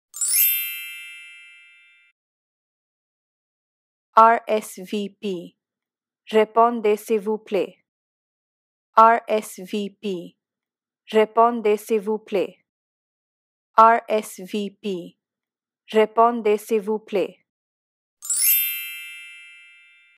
How to Pronounce RSVP
Learn how to pronounce the phrase 'RSVP' with Unstammer's free pronunciation tutorials. Also read the meaning(s) and usage of RSVP